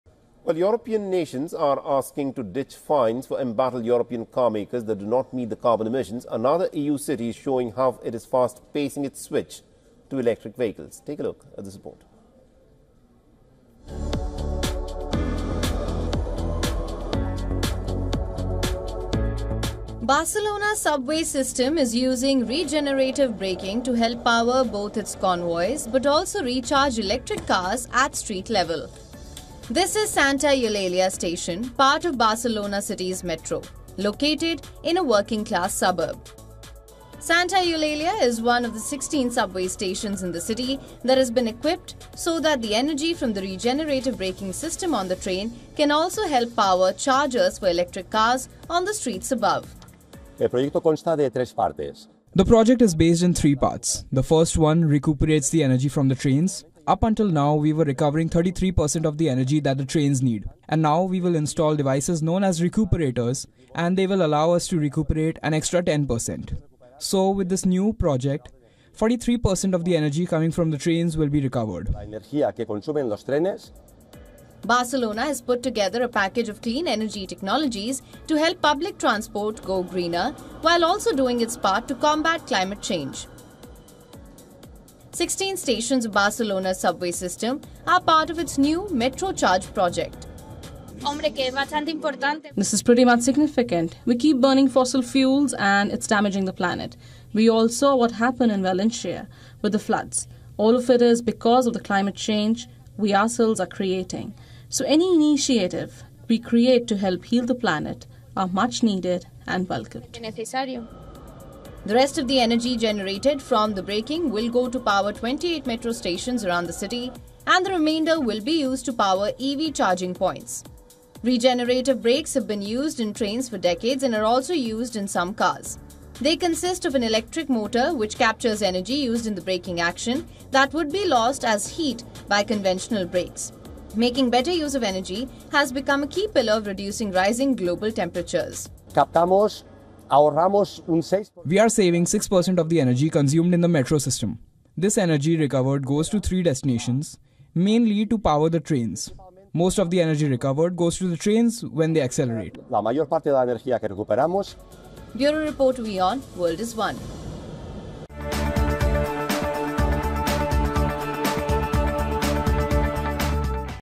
Spain Shows How To Cut Car Emissions; Regenerative Braking Powers Subways & EV Chargers | WION
Barcelona is turning its subway trains into power stations, harnessing energy from braking systems to generate electricity, as part of the city's push towards sustainability and green energy. Watch in for more details!